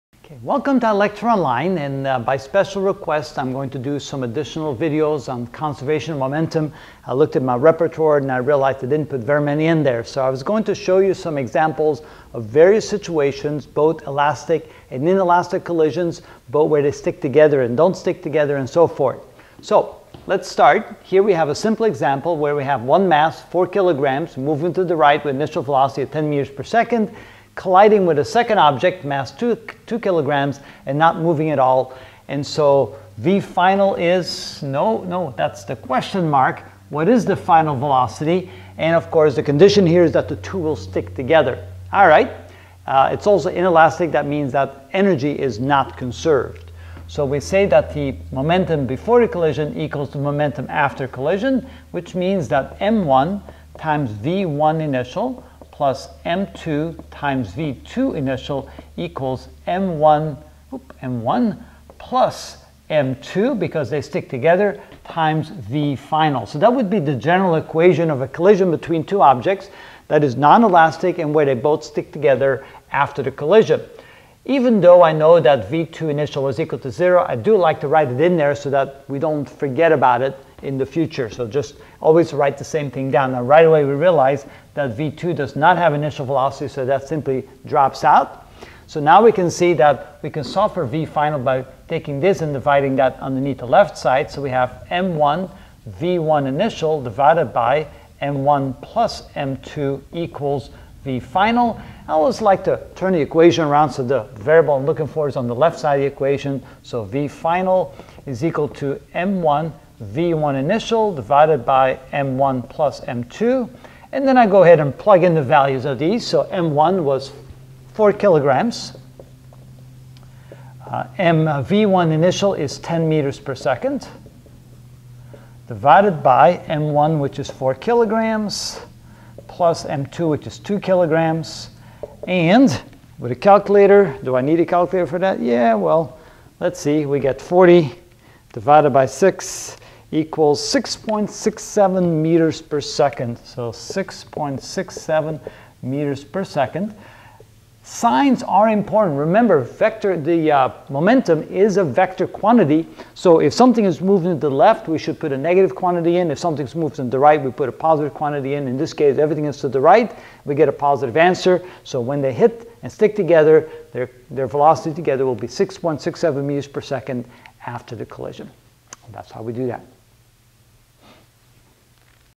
Physics 10 Momentum and Impulse (16 of 30) Inelastic Collision Ex.1
In this video I will find velocity final of an inelastic collision, example 1.